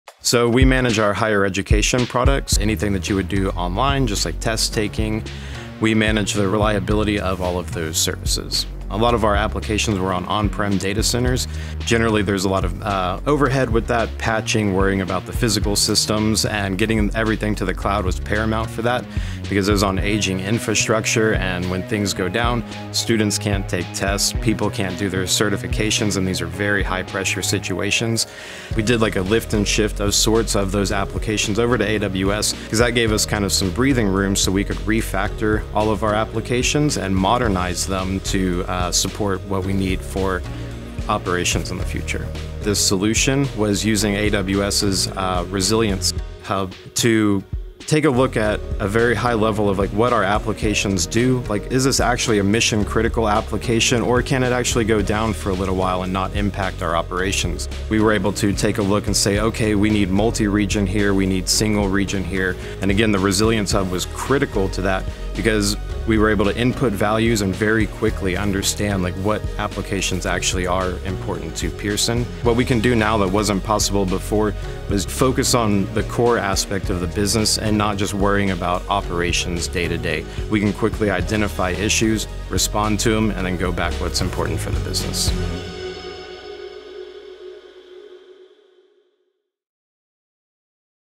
Pearson taps AWS Resilience Hub to improve application resilience | Amazon Web Services
Hear from Ronnie Kendrick, Senior SRE Manager for Infrastructure and Operations at Pearson, about how Pearson leveraged AWS Resilience Hub to assess the resilience of their applications, make informed decisions about application criticality, and improve their overall resilience posture.